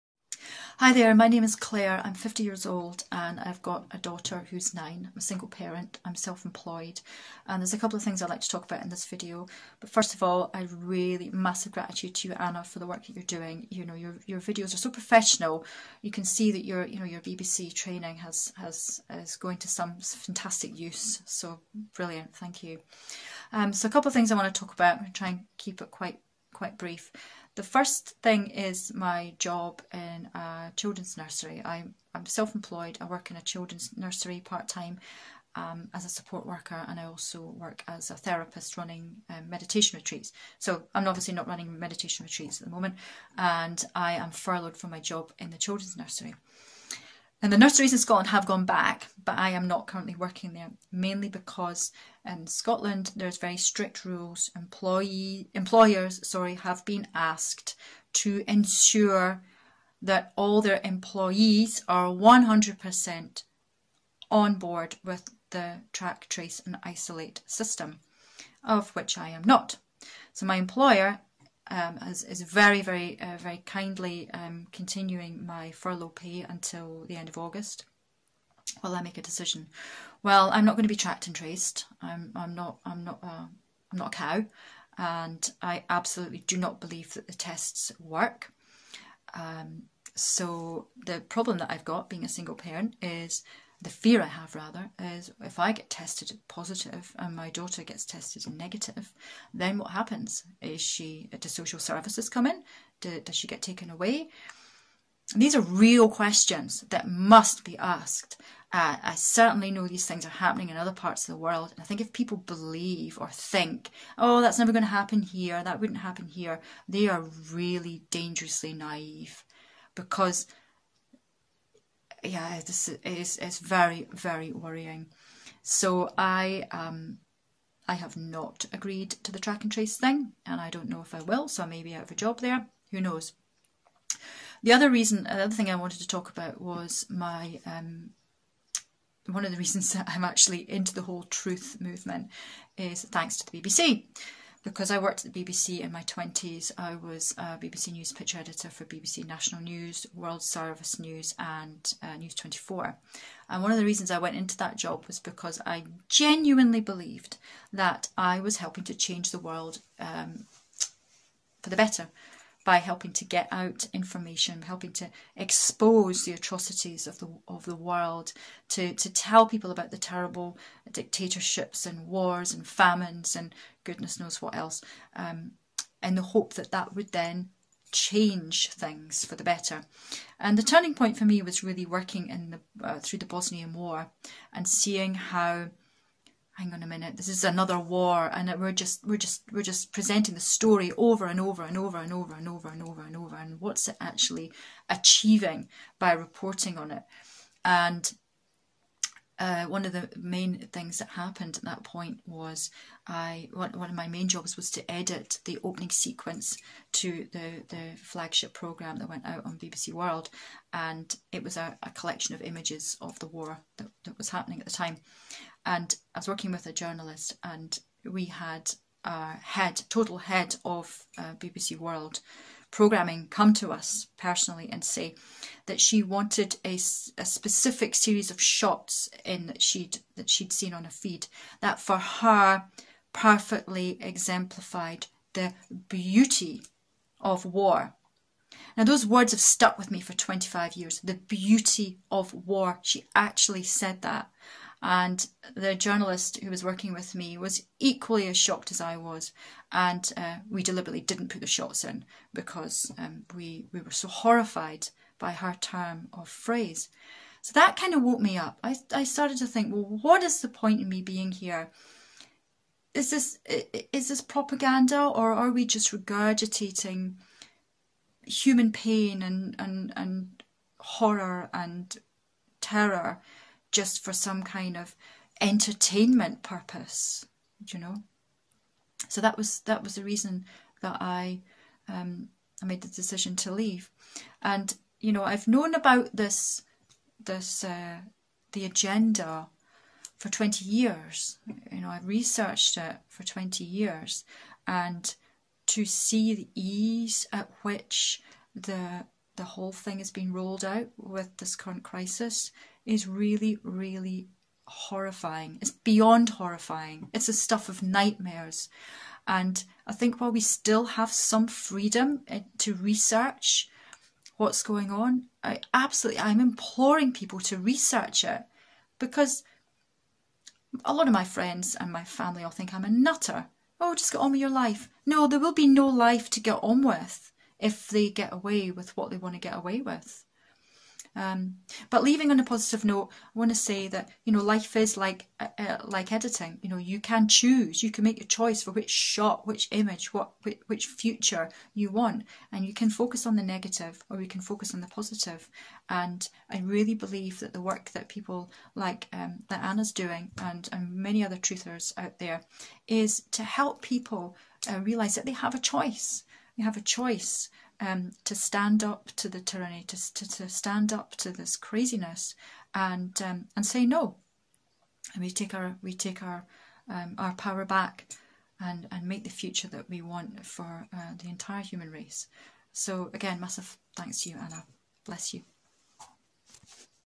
"The Mood of Britain" #3, July 2020 (mirrored for Extinction Rebellion)
Claire, 50, Single Parent, Ex BBC Picture Editor 26 July 2020.